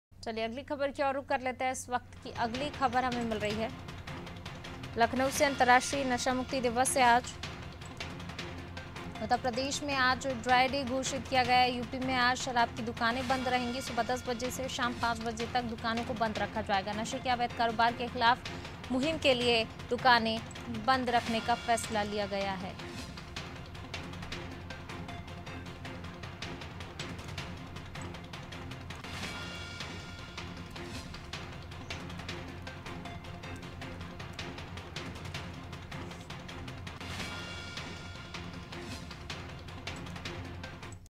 Lucknow :अंतर्राष्ट्रीय नशा मुक्ति दिवस आज | Uttar Pradesh News Hindi
लखनऊ :अंतर्राष्ट्रीय नशा मुक्ति दिवस आज
उत्तरप्रदेश में आज ड्राई-डे घोषित
यूपी में आज बंद रहेंगी शराब की दुकानें
सुबह 10 से शाम 5 बजे तक बंद रहेंगी दुकानें
नशे के अवैध कारोबार के खिलाफ मुहिम के लिए बंद